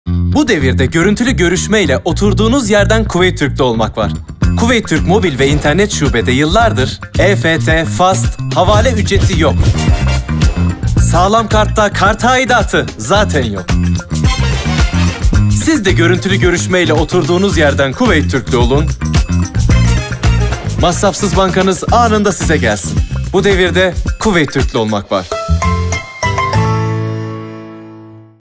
Görüntülü Görüşmeyle Oturduğunuz Yerden Kuveyt Türklü Olun!
Bu devirde görüntülü görüşmeyle oturduğunuz yerden Kuveyt Türklü olmak var. Kuveyt Türk Mobil'i indirerek dilediğiniz yerden, dilediğiniz zamanda görüntülü görüşmeyle anında Kuveyt Türklü olun, masrafsız bankacılığın keyfini çıkarın.